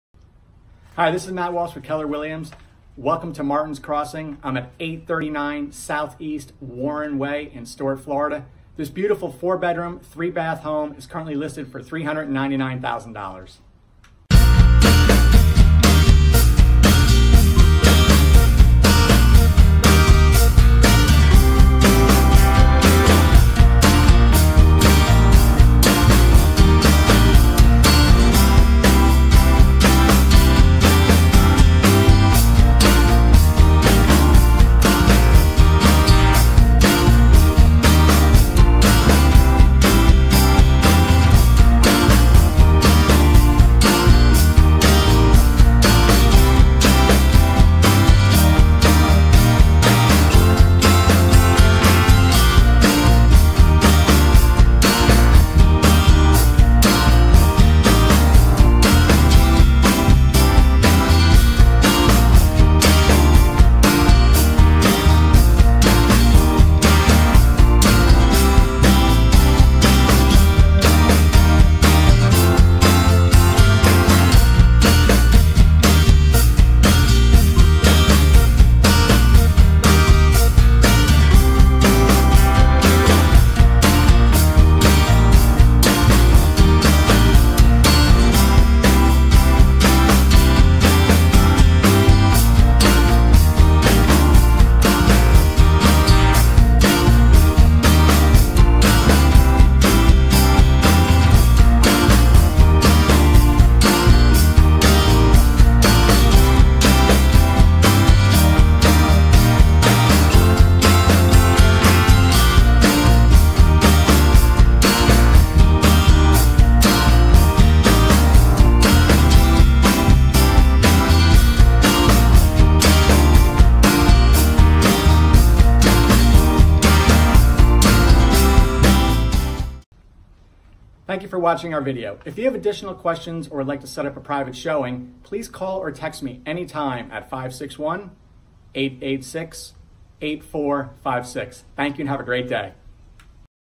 4 bedroom 3 bath home\tMartins Crossing Stuart Florida 34997 With pool
Martins Crossing is a Family Friendly community close to major roadways, shopping and dining as well as festivals and waterfront activities. Down town Stuart is minutes away! Excellent schools ! Soaring ceilings greet you as soon as you enter and this home. It is light and bright! Large eat in kitchen as well as formal dining area, formal living room AND family room allows plenty of space for friends and family! The patio / pool area has plenty of room for entertaining as well.